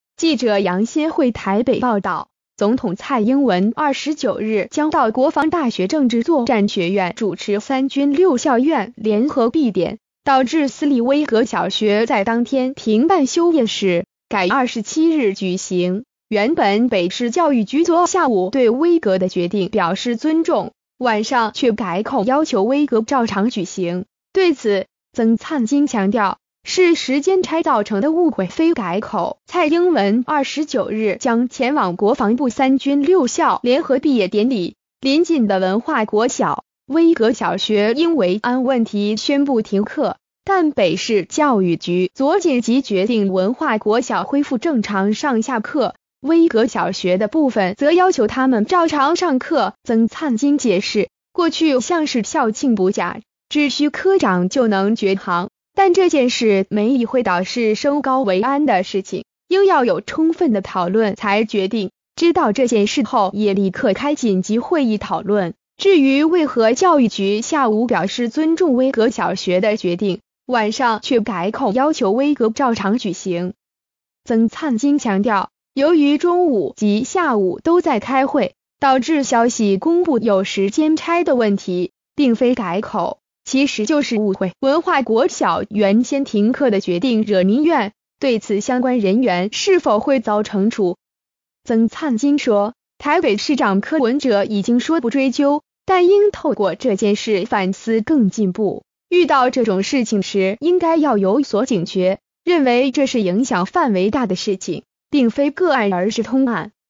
薇閣停課問題 北市教局：時間差造成誤會非改口
〔記者楊心慧／台北報導〕總統蔡英文29日將到國防大學政治作戰學院主持三軍六校院聯合畢典，導致私立薇閣小學在當天停辦休業式，改27日舉行，原本北市教育局昨下午對薇閣的決定表示尊重，晚上卻改口要求薇閣照常舉行，對此，曾燦金強調，是時間差造成的誤會非改口。

發表評論評論如下。